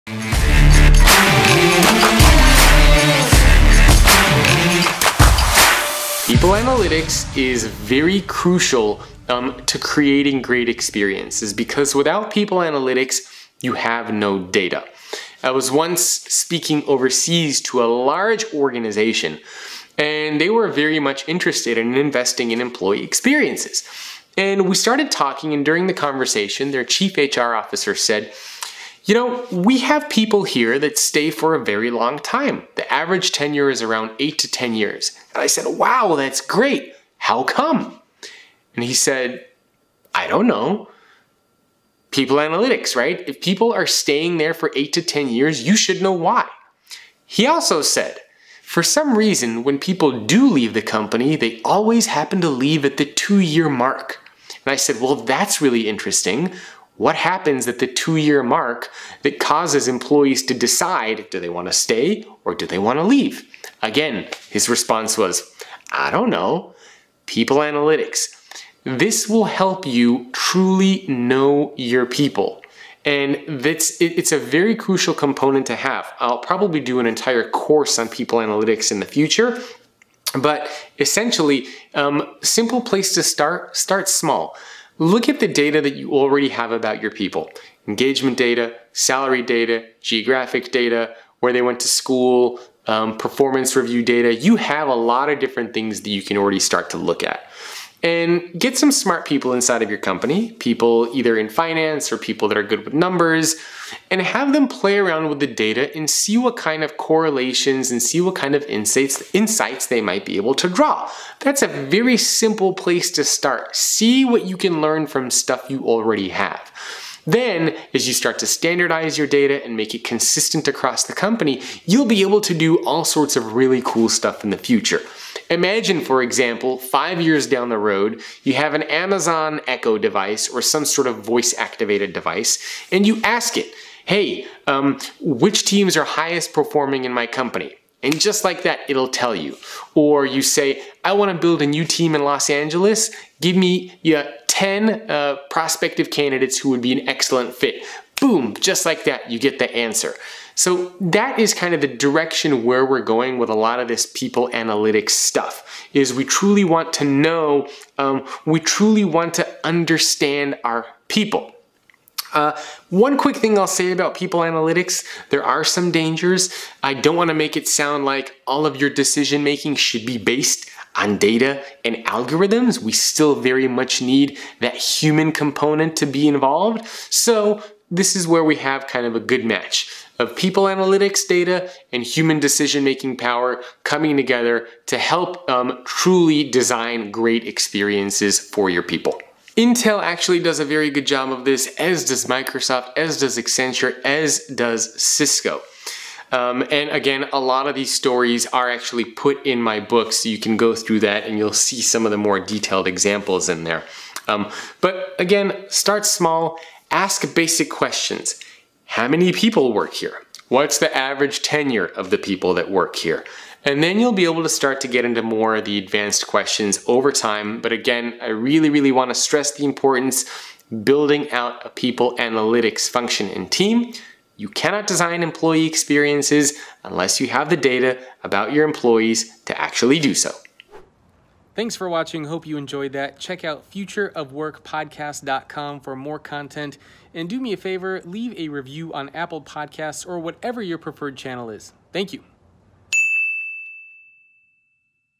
Do You Need People Analytics?
People analytics is crucial to creating great experiences. Without people analytics, you don’t have data. And data is what helps you truly know your people.

I was once speaking overseas to a large organization that was very interested in investing in employee experiences. The Chief HR Officer said that the average tenure in the organization was eight to ten years. I said, “Wow, that’s great. How come?” And he said, “ I don’t know”.

He also said, “When people do leave the company, they always leave at the two-year mark.” But when I asked what happens at the two-year mark, he said he didn’t know.

Look at the data you already have about your people--engagement data, salary data,
geographic data, where they went to school, performance review data. You have a lot of ways to find data and learn about your employees.